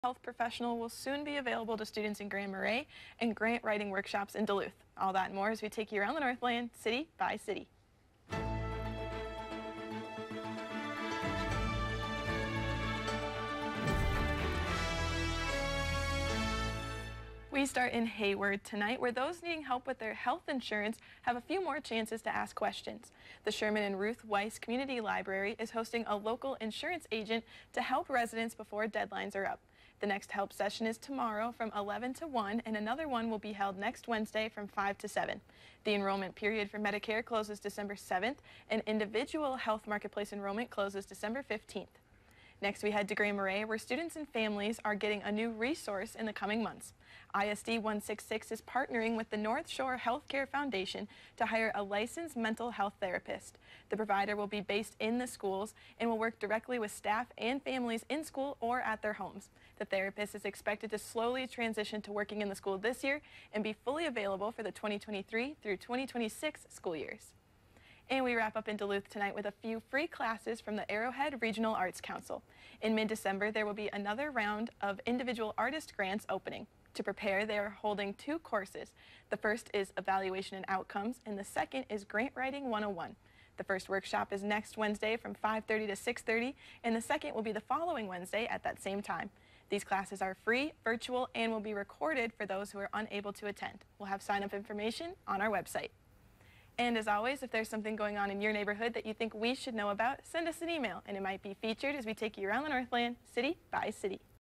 City by City: Hayward, Grand Marais, Duluth
The Sherman and Ruth Weiss Community Library is hosting a local insurance agent to help residents before deadlines are up.